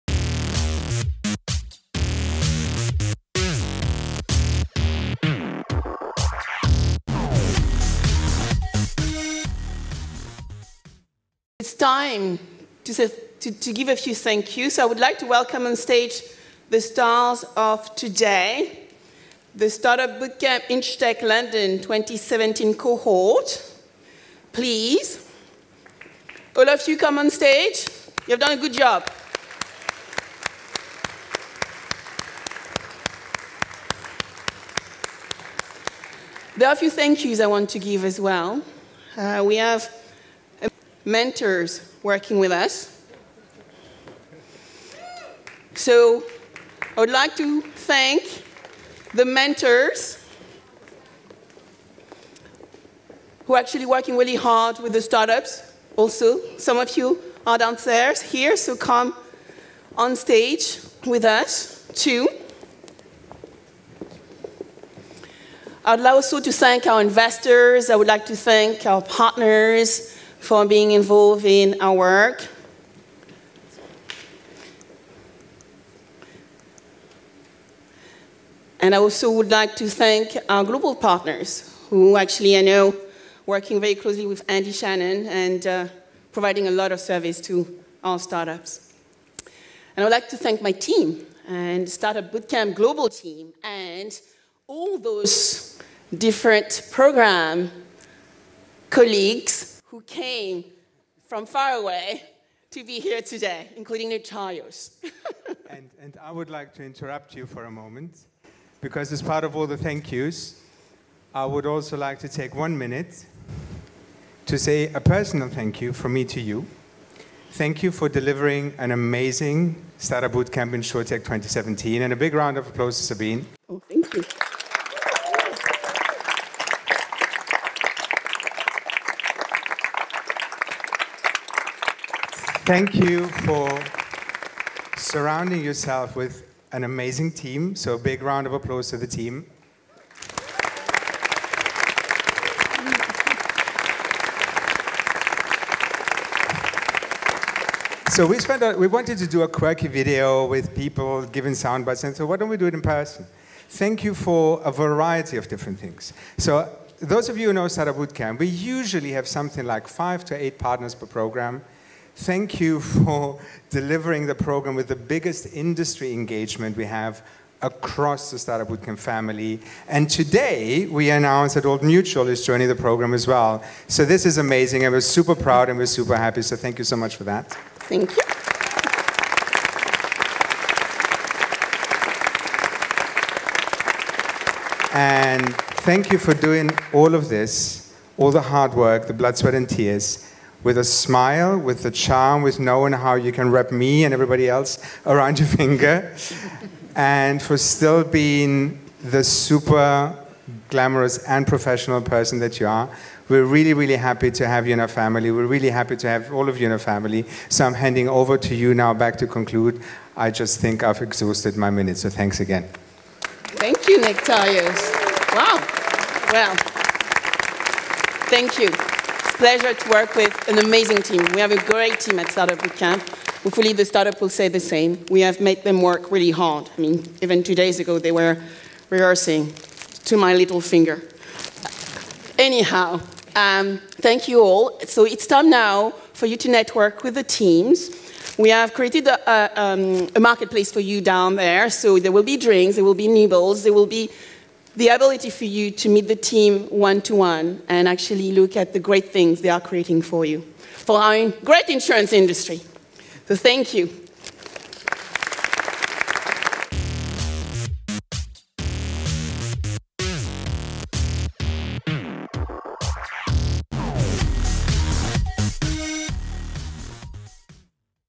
Startupbootcamp InsurTech Demo Day 2017 - Closing Remarks
On the 26th of April, at Indigo at The O2 in London, 10 startups and 1 mature Startup in Residence explained how they're changing insurance to a room of over 850 people, including insurers, investors, mentors, and the insurance technology community.